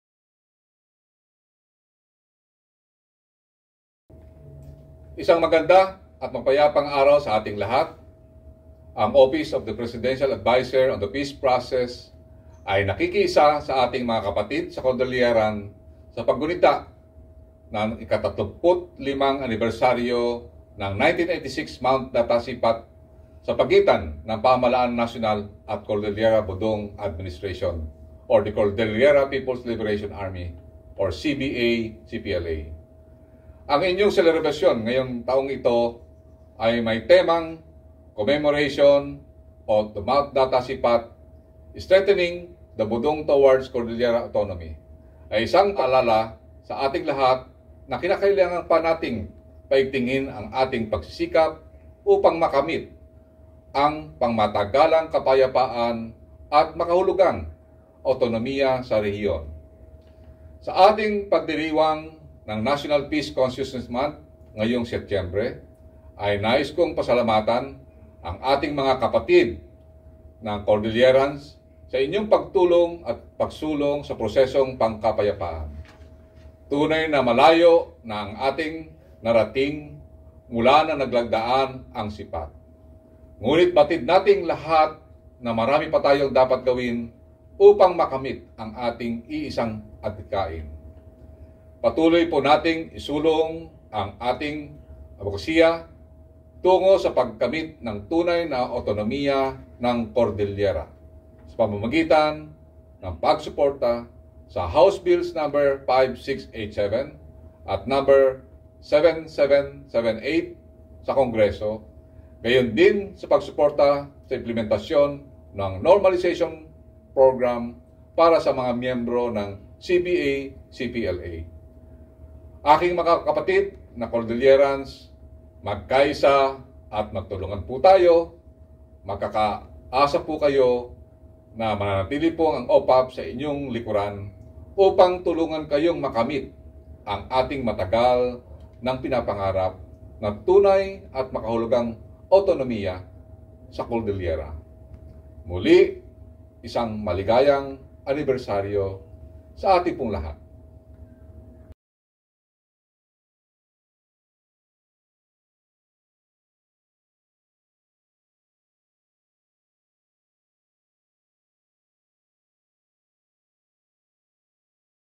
Celebration of 35th Anniversary of the 1986 SIPAT
Message of Presidential Peace Adviser Carlito G. Galvez, Jr. on the 35th Anniversary of the 1986 SIPAT with the Cordillera Bodong Administration - Cordillera Peoples' Liberation Army (CBA-CPLA)

13 September 2021